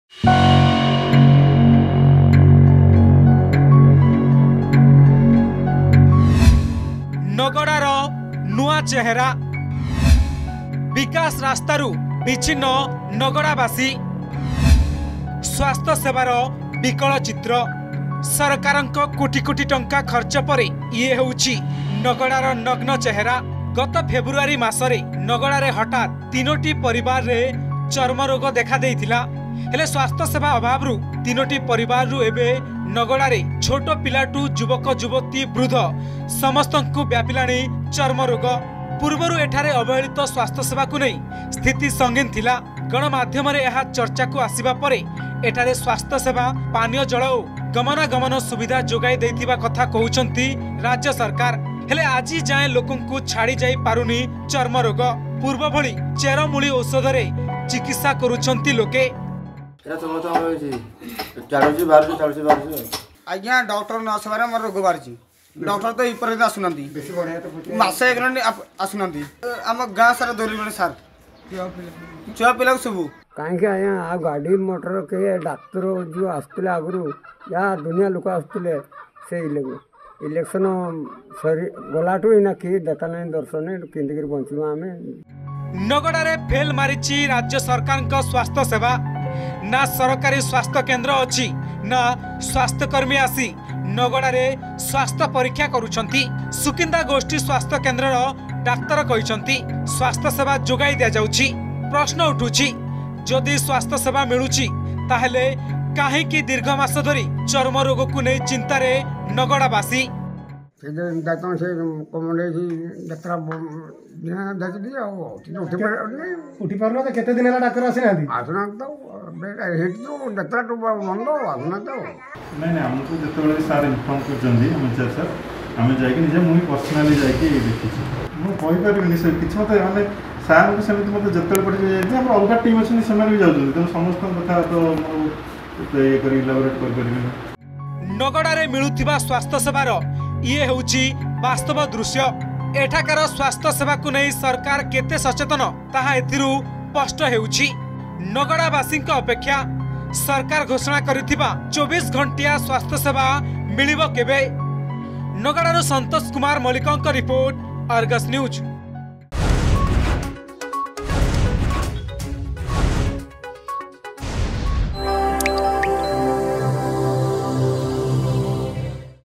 Jajpur: No Health Care Facility in Nagada Village due to Negligence of Odisha Govt
Due to the negligence of the Odisha government, there is no health care facility in Nagada Village, Jajpur district. Three families in this village have been diagnosed with fungal infection, which is spreading to other villagers due to a lack of treatment. Still Odisha Government's health department takes no action regarding this.